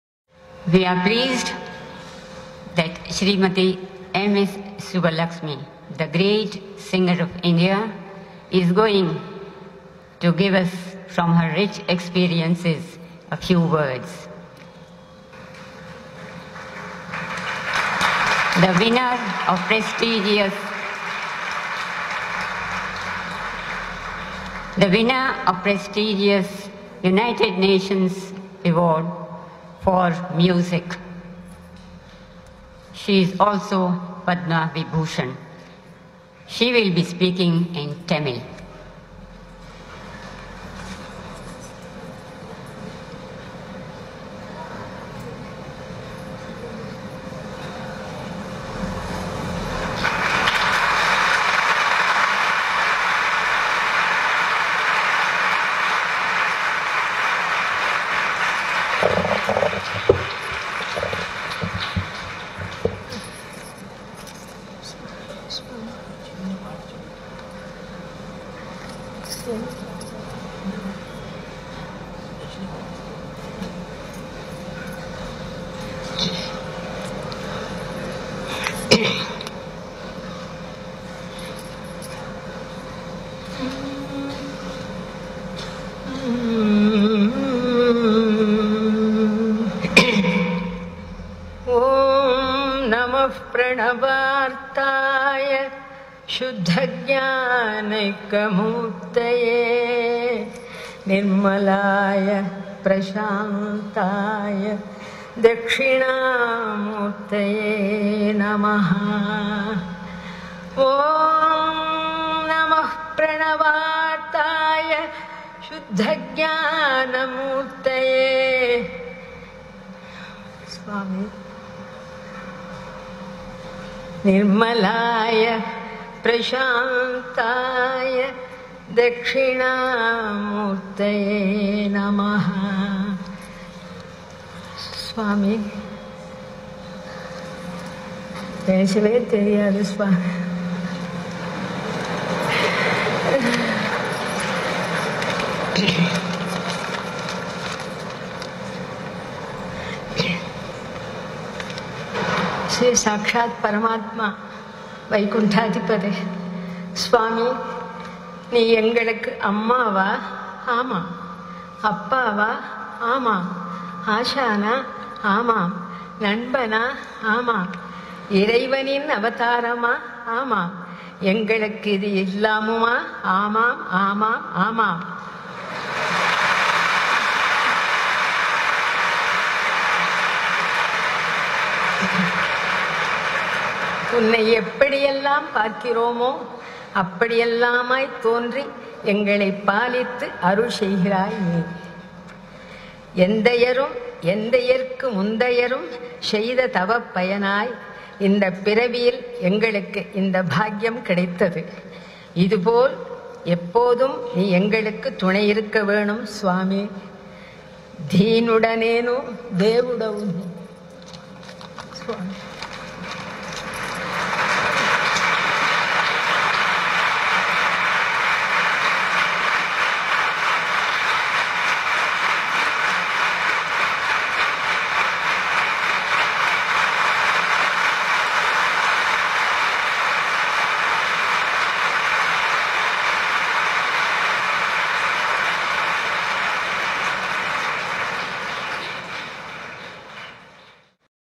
TU SEI TUTTO PER NOI- VIDEO DOLCISSIMO-
Una famosa cantante indiana dedica a Swami
delle Lodi. dolciissima!

Immagini e video tratte da Radio Sai Global Harmony